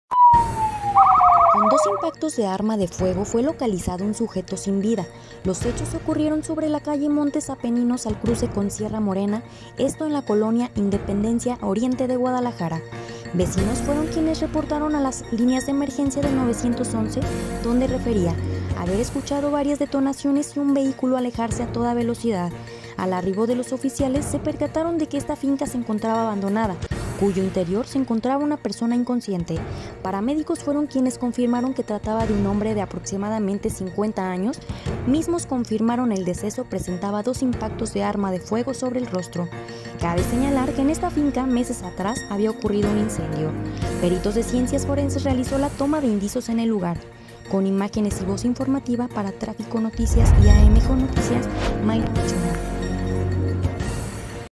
Con dos impactos de arma de fuego fue localizado un sujeto sin vida.
Los hechos ocurrieron sobre la calle Montes Apeninos al cruce con Sierra Morena, en la colonia Independencia Oriente .